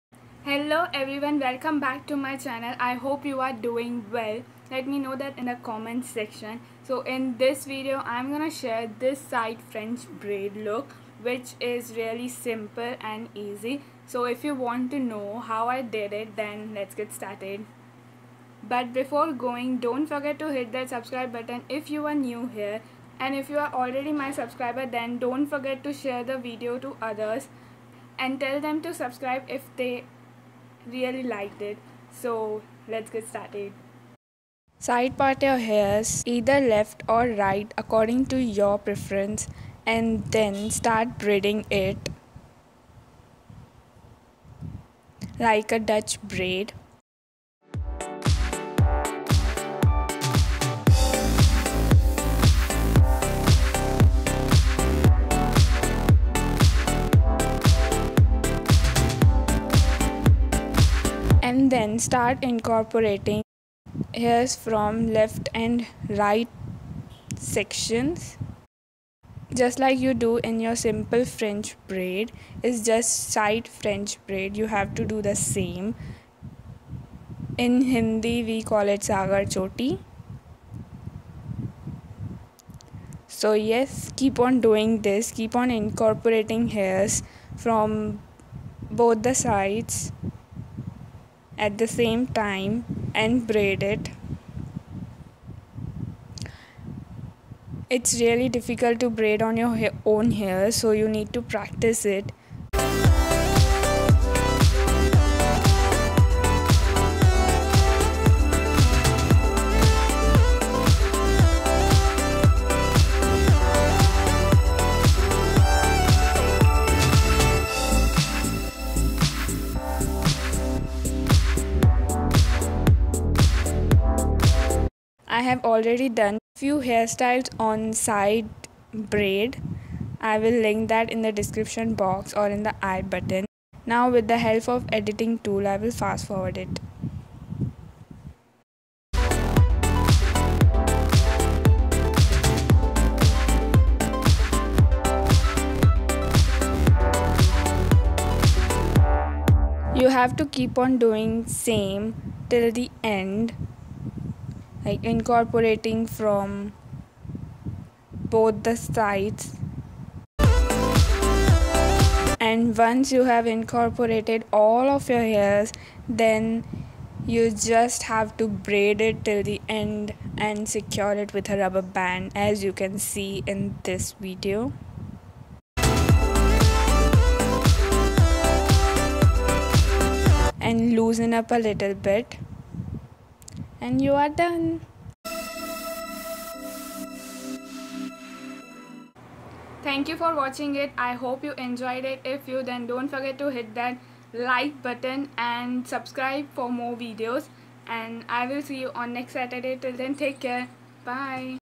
5 mins Side French Braid Hairstyle Tutorial | How to
Today I'm going to share a tutorial on Side French Braid which won't take more than 5 mins

Happy Caring..

Love,
XOXO
Vandhana